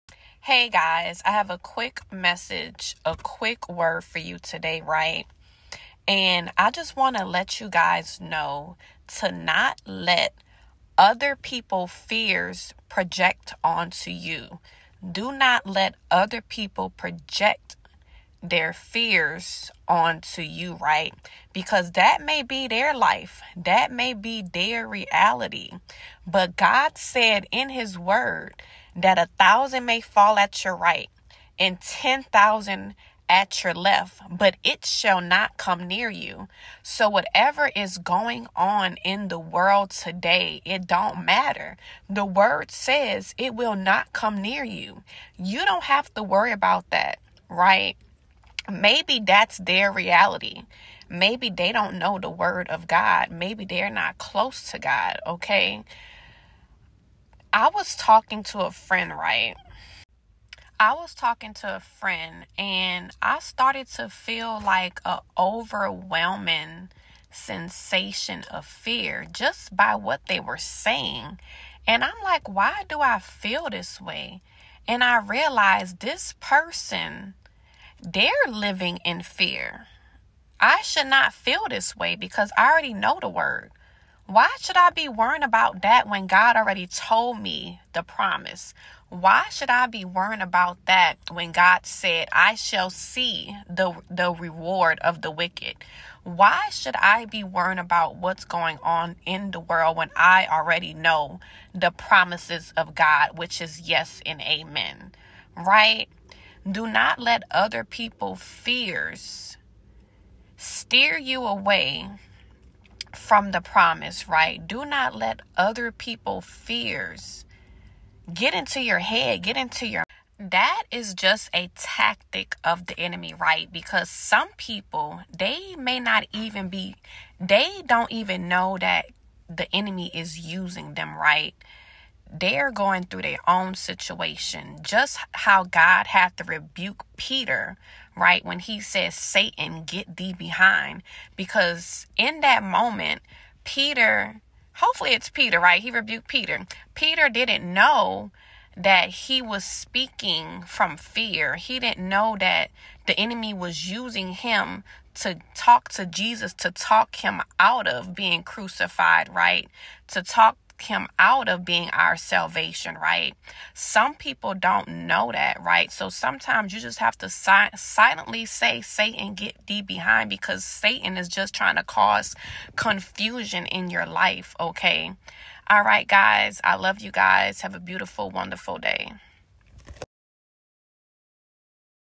DON’T let THEM do THIS CHILD of GOD 😰
Don’t let others project their fears onto you

Matthew 16:24

Psalm 91:7

A thousand may fall at your side, ten thousand at your right hand, but it will not come near you.

2 Corinthians 1:20

“For no matter how many promises God has made, they are 'Yes' in Christ. And so through him the 'Amen' is spoken by us to the glory of God”